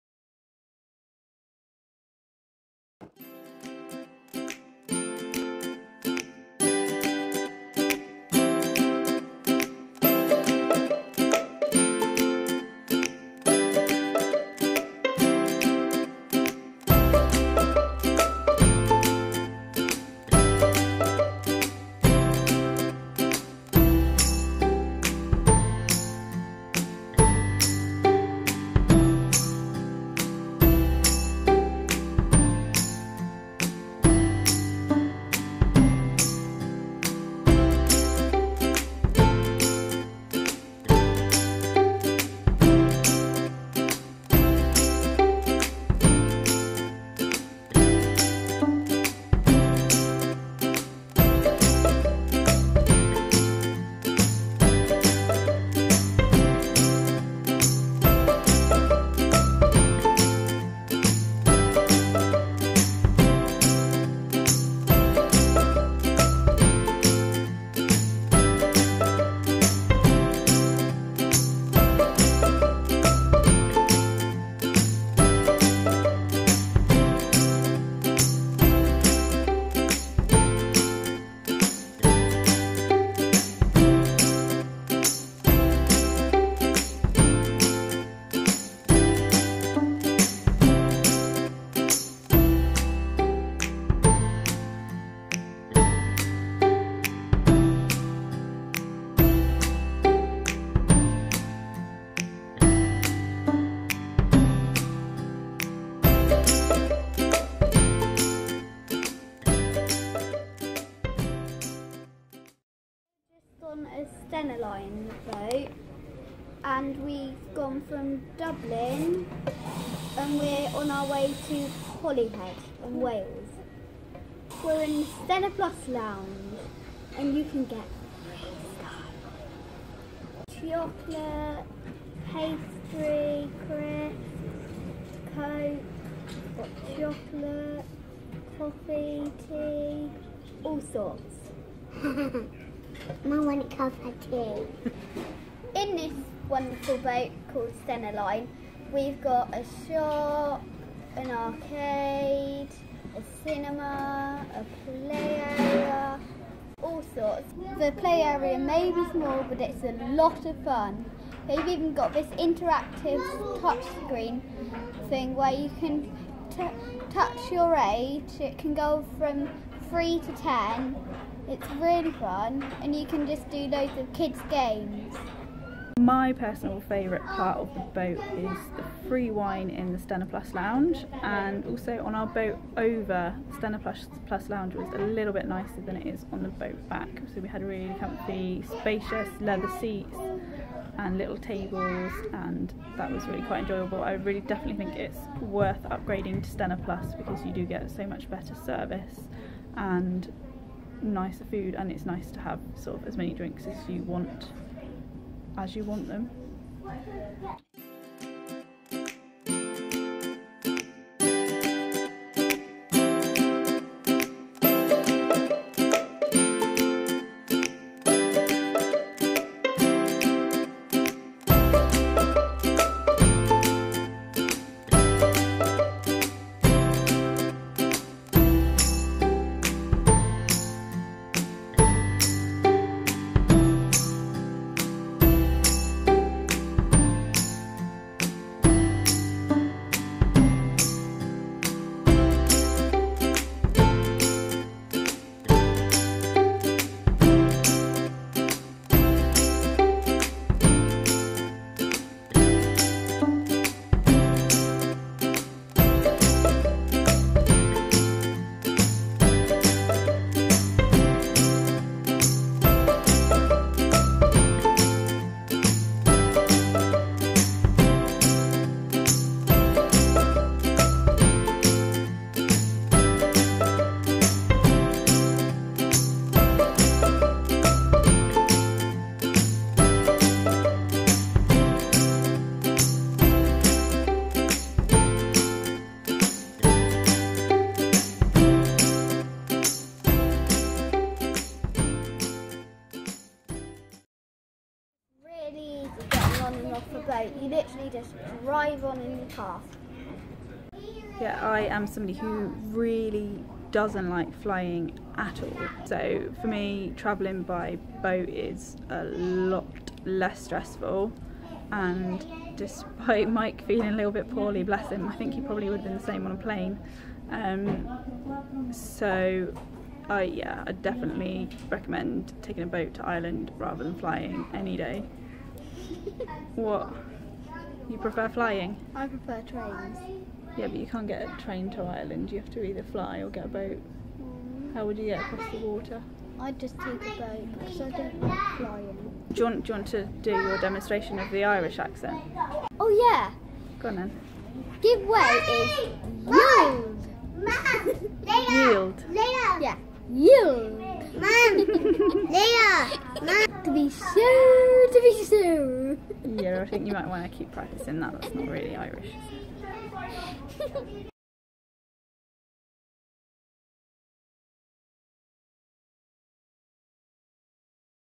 Stena Line Plus Dublin to Holyhead
Stena Line Review. Holyhead to Dublin and Dublin to Holyhead. Footage filmed over both crossings on two different boats.